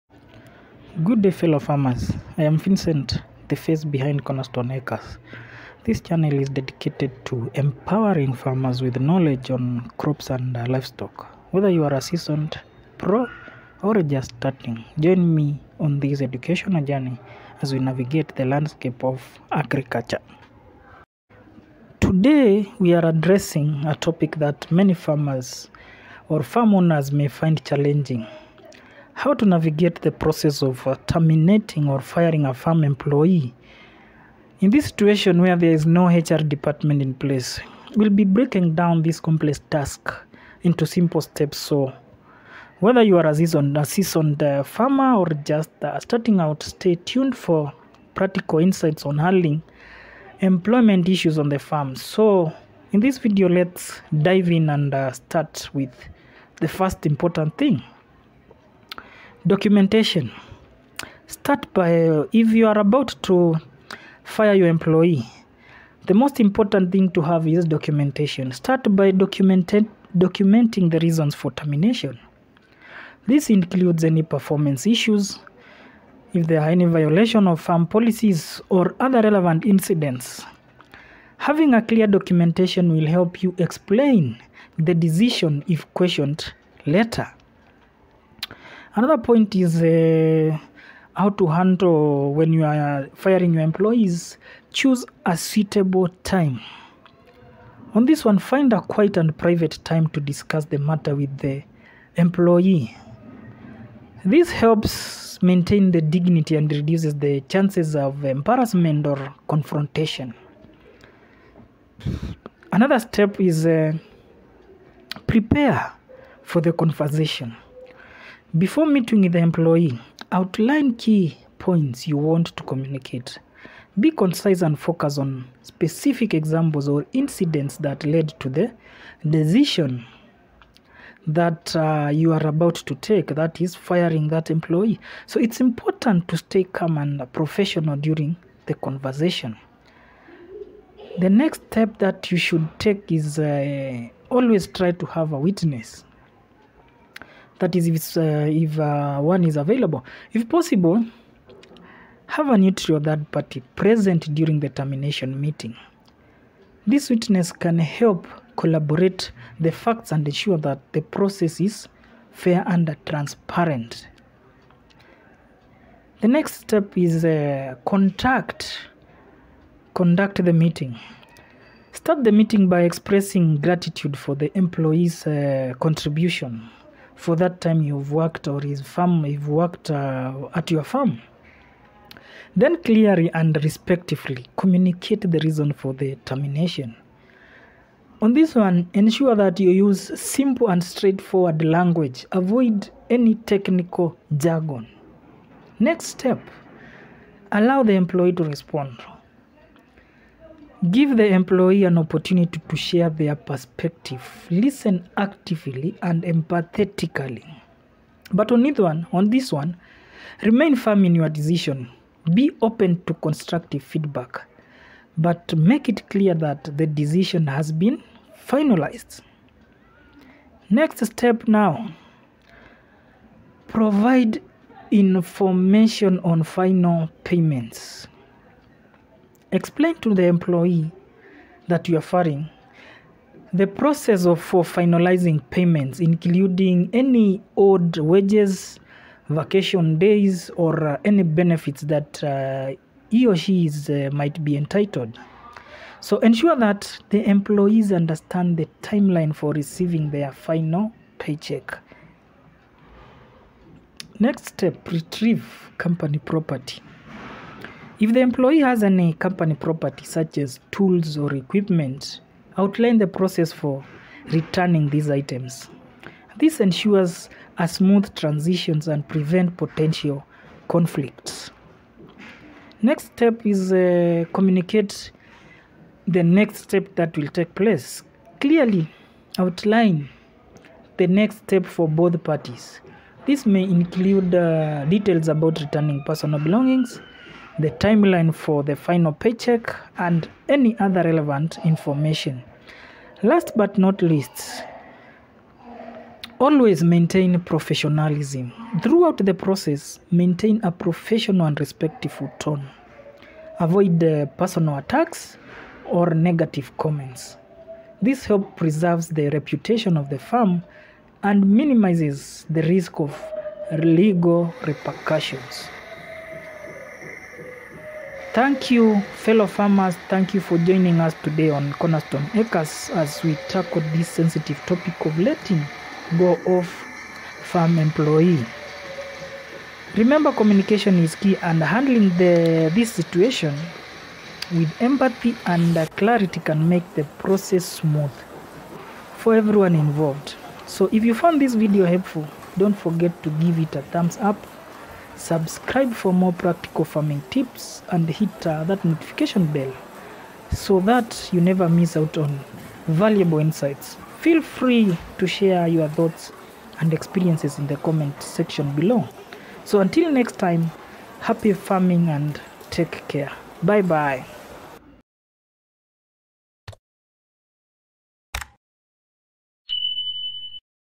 Firing Farm Employees with Respect; DIY Termination Tips
It’s a conversation no one wants to have, but is sometimes necessary. Firing an employee is far from easy, but with some prior preparation, it can be less stressful for both the employer and employee.